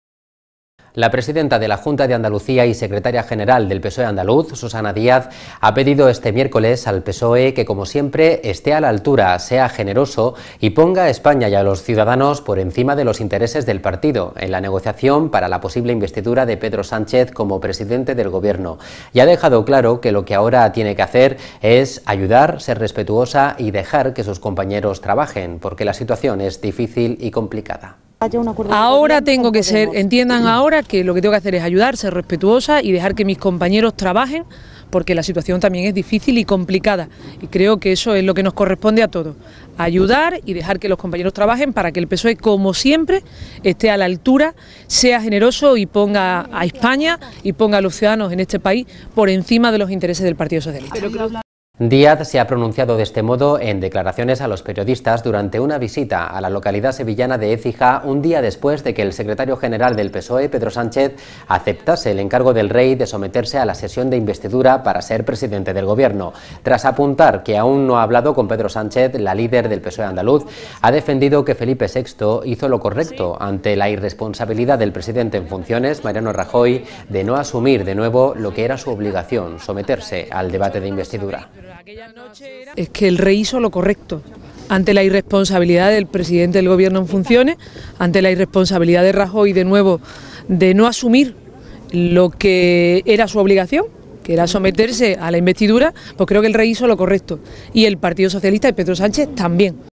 Susana Díaz pide poner a España "por encima del interés del PSOE"
Español:
La presidenta de la Junta de Andalucía y secretaria general del PSOE-A, Susana Díaz, ha pedido este miércoles al PSOE que, "como siempre, esté a la altura, sea generoso y ponga a España y a los ciudadanos por encima de los intereses del partido" en la negociación para la posible investidura de Pedro Sánchez como presidente del Gobierno y ha dejado claro que lo que ahora tiene que hacer es ayudar, ser respetuosa y dejar que sus compañeros trabajen, porque la situación es difícil y complicada.

Díaz se ha pronunciado de este modo en declaraciones a los periodistas durante una visita a la localidad sevillana de Écija un día después de que el secretario general del PSOE, Pedro Sánchez, aceptase el encargo del Rey de someterse a la sesión de investidura para ser presidente del Gobierno.

Tras apuntar que aún no ha hablado con Pedro Sánchez, la líder del PSOE andaluz ha defendido que Felipe VI "hizo lo correcto" ante la "irresponsabilidad" del presidente en funciones, Mariano Rajoy, "de no asumir de nuevo lo que era su obligación, someterse al debate de investidura".

English:
The President of Andalusia and general secretary of the PSOE-A, Susana Diaz, asked Wednesday the PSOE that "as always, falls short, be generous and put Spain and citizens above the interests of party "in negotiating the possible inauguration of Pedro Sanchez as prime minister and has made clear that what we now need to do is to help, to be respectful and let your colleagues to work, because the situation is difficult and complicated.

Diaz has spoken thus told reporters during a visit to the Seville town of Ecija a day after the general secretary of the PSOE, Pedro Sanchez, commissioned by the King accepted to undergo the investiture session to be president of the government.

After pointing out that he has not yet spoken to Pedro Sanchez, the leader of the Andalusian PSOE has defended Philip VI "did the right thing" to the "irresponsibility" of the acting president, Mariano Rajoy, "not to take back what was his duty , submit to the investiture debate. "